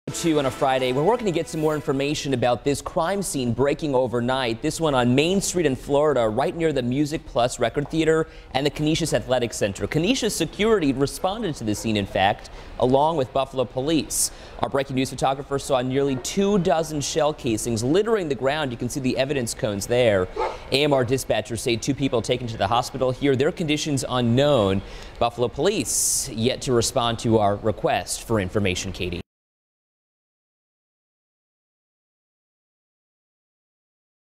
Two people taken to hospital in apparent shooting near Canisius
Buffalo police are investigating overnight shooting near Canisius College.  Two people were sent to the hospital.